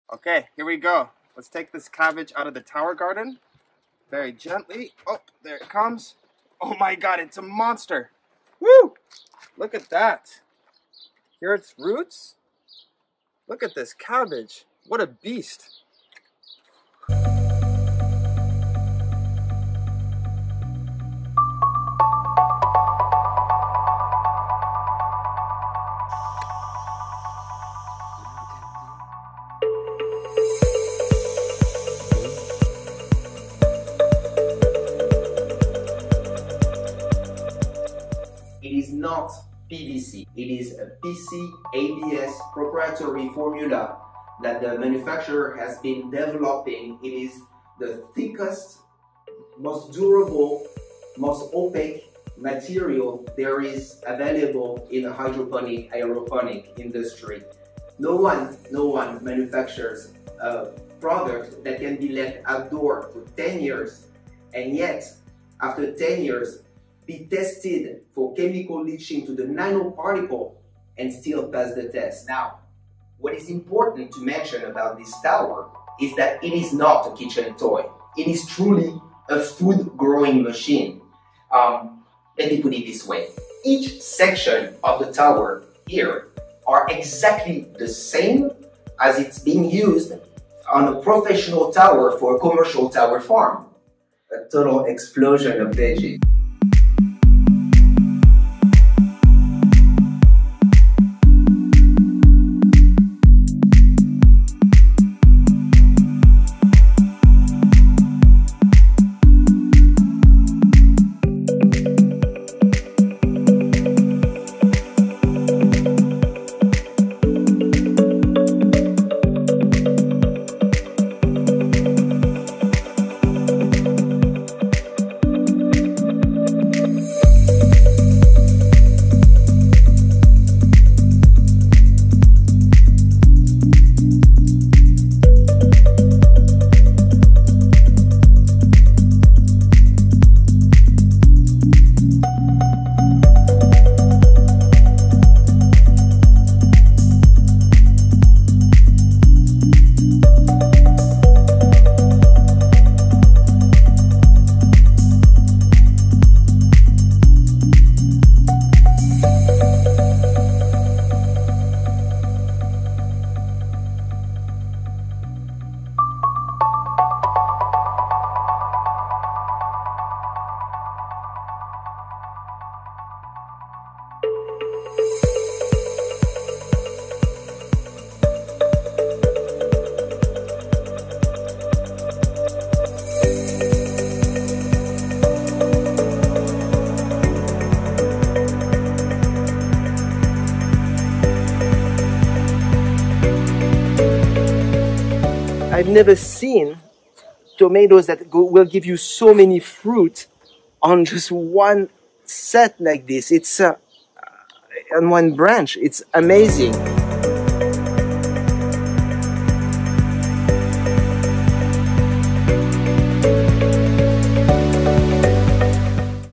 Vertical Farming with Aeroponic Tower Gardens
Vertical farming with aeroponic Tower Garden systems allows you to grow over 200 different crops including vegetables, herbs, fruit, leafy greens, and edible/ornamental flowers. Learn how to grow aeroponic food!